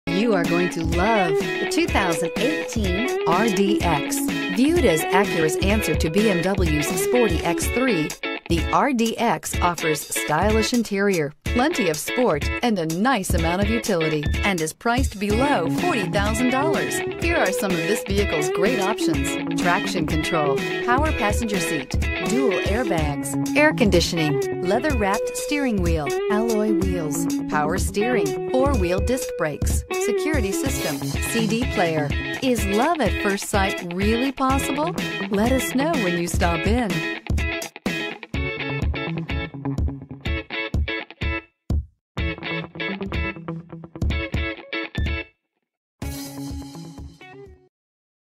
2018 Acura RDX Culver City CA 18A0275
2018 Acura RDX Base - $36059

Nissani Brothers Acura
5750 Mesmer Ave

New Price! Lunar 2018 Acura RDX FWD 6-Speed Automatic 3.5L V6 SOHC i-VTEC 24Vbrbr28/20 Highway/City MPG**